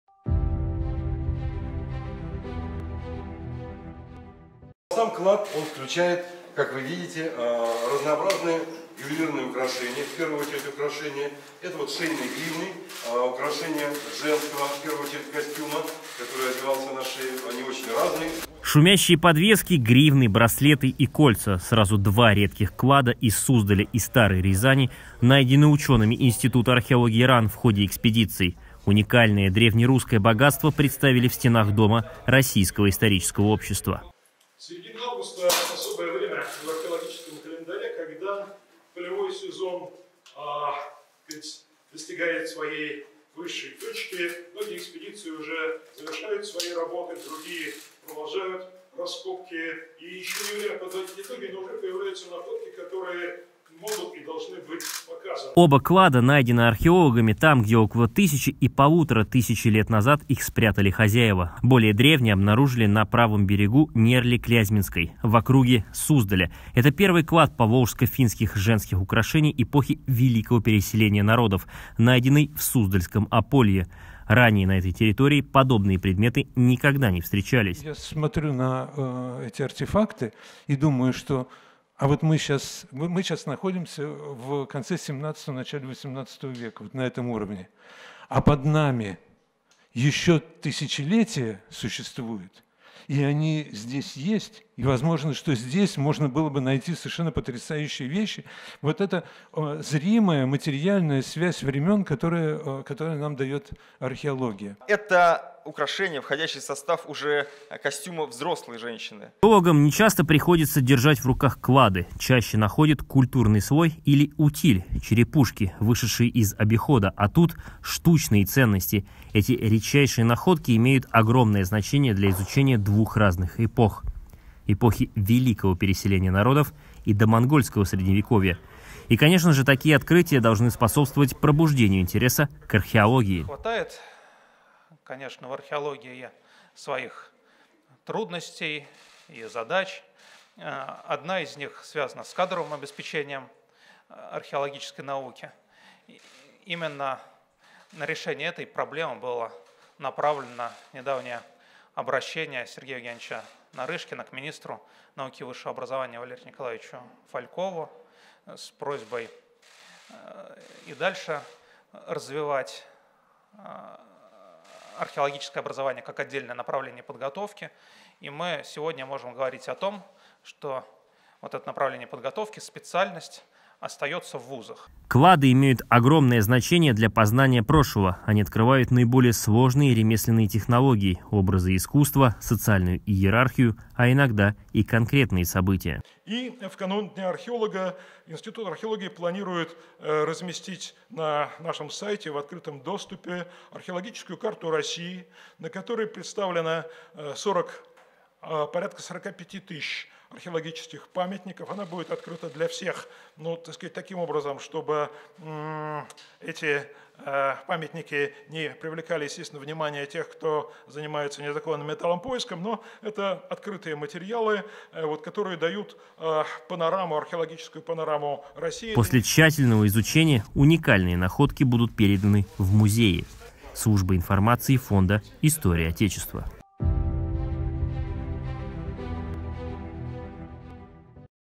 Институт археологии РАН провёл презентацию кладов «Археологические жемчужины 2021 года»
13 августа в преддверии Дня археолога в Доме Российского исторического общества прошла презентация Института археологии РАН «Археологические жемчужины 2021 года». Учёные представили два уникальных клада разных эпох.

Оба клада представляют собой набор ювелирных украшений, входящих в состав женского костюма. Клад, найденный в Суздальском Ополье, состоит из комплекта поволжско-финских украшений середины I тысячелетия. А тот, что был обнаружен в округе городища Старая Рязань, датируется первой половиной — серединой XII века и содержит серебряные шейные гривны, браслеты, височные кольца.

Клады были обнаружены in situ — там, где их оставили хозяева около тысячи лет назад. Тщательная фиксация всех предметов клада и изучение культурного слоя позволят археологам воссоздать исторический контекст и понять: когда, как и при каких обстоятельствах был спрятан клад, а также нарисовать примерный портрет его хозяина.

О комплекте поволжско-финских украшений, обнаруженном Суздальской экспедицией ИА РАН на правом берегу Нерли-Клязьминской в округе Суздаля, рассказал Александр Сергеевич Морозов, младший научный сотрудник отдела средневековой археологии ИА РАН.

Клад оказался объёмным: не только набор украшений женского традиционного костюма, но и импортная металлическая чаша, которая могла иметь ритуальное значение. Находки выполнены в технике литья по утрачиваемой восковой модели, поэтому являются уникальными. Каждый предмет клада — яркий образец поволжско-финского ювелирного дела, обладающий неоспоримой художественной ценностью.